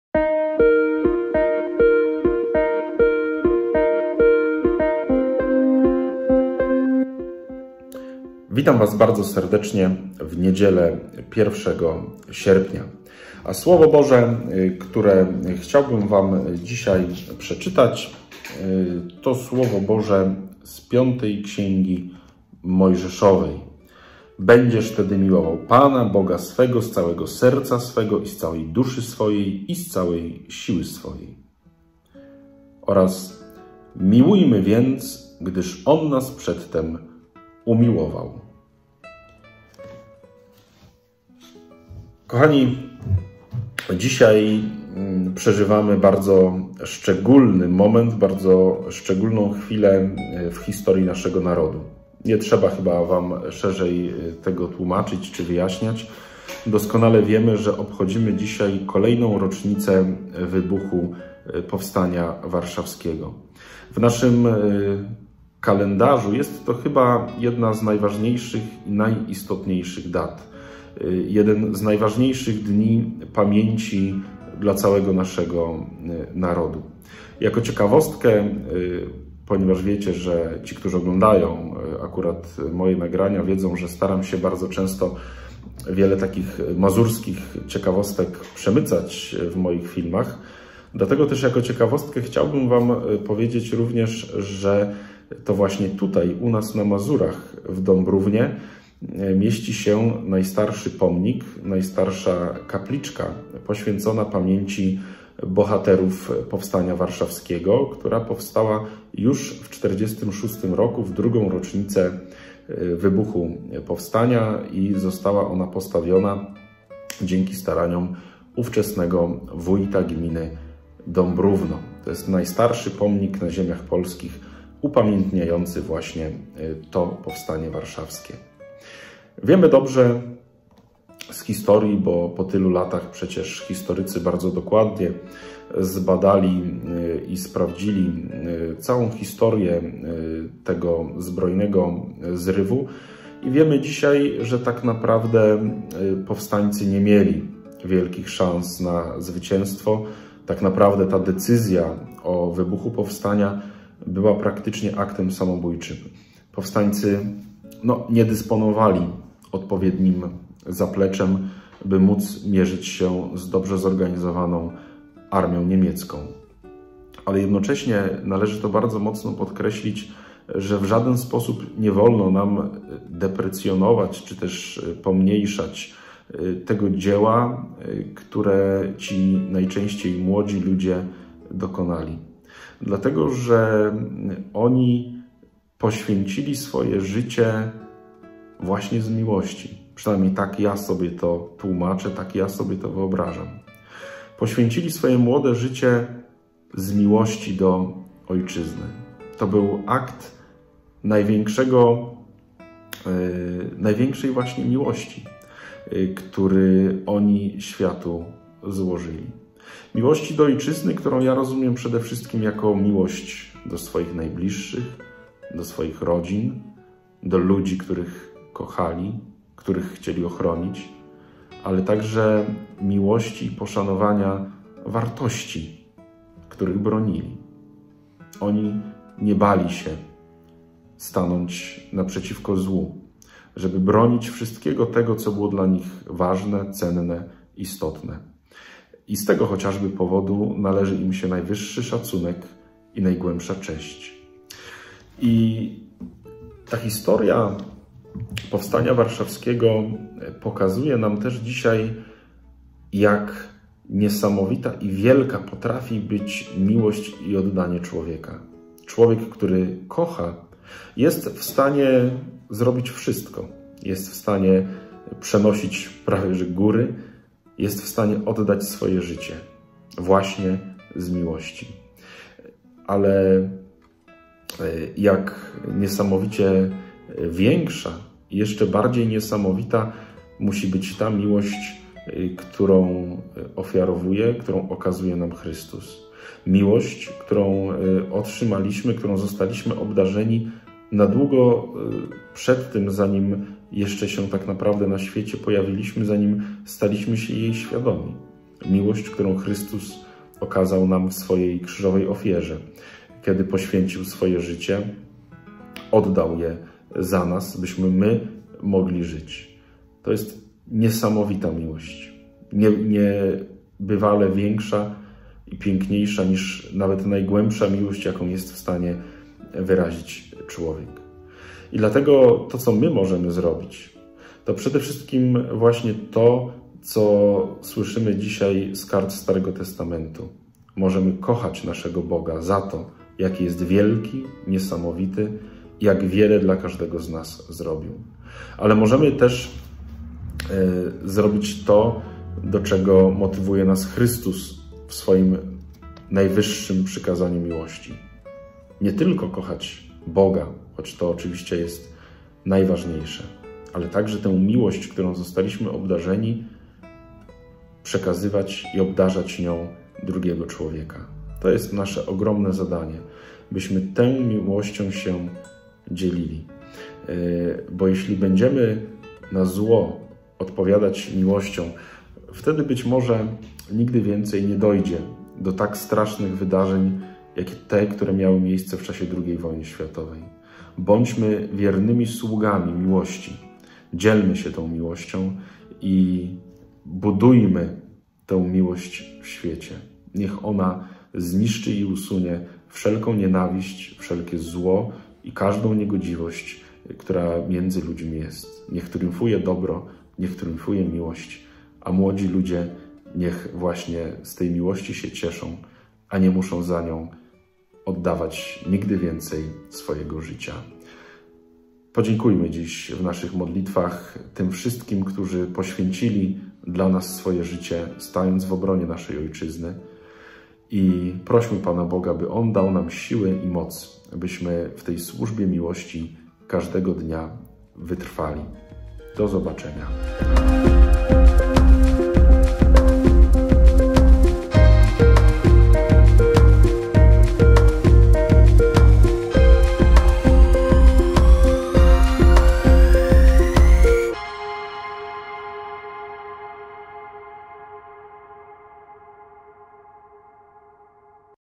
Codziennik biblijny, Słowo na dzień 1 sierpnia 2021 r.
ks. Wojciech Płoszek, Parafia Ewangelicko-Augsburska w Ostródzie
Rozważanie biblijne na Słowo dnia:

Będziesz tedy miłował Pana, Boga swego, z całego serca swego i z całej duszy swojej, i z całej siły swojej.
Piąta Księga Mojżeszowa 6,5

Miłujmy więc, gdyż On nas przedtem umiłował.
Pierwszy List Jana 4,19
Codziennie o 7 rano spotkanie z Biblią.